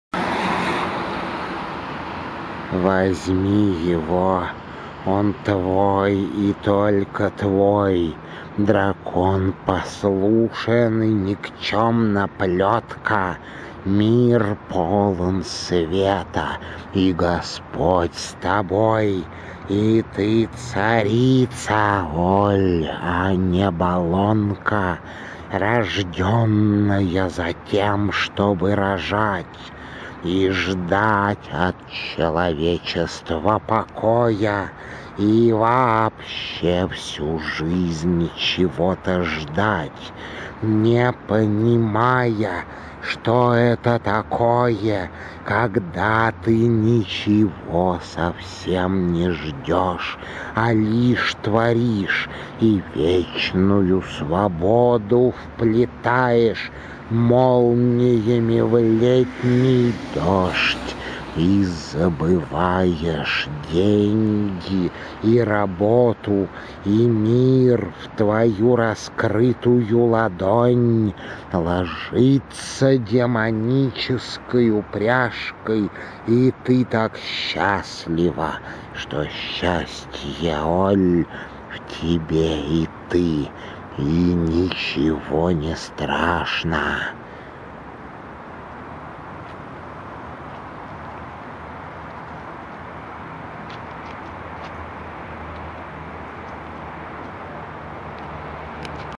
11-03-2011 - отдание Георгием поверженного дракона Ольге
Москва, 2011

снялось и прочиталось по случаю

полное название: отдание святым великомучеником Георгием поверженного дракона святой равноапостольной княгине Ольге

стихи написаны порядком уже тому назад... пытался их читать, но впервые как-то вышло внятно... простые на вид и непростые для автора оказались стихи... нашёл рисунок на асфальте мокром рядом с фонарём подходящий, как увиделось...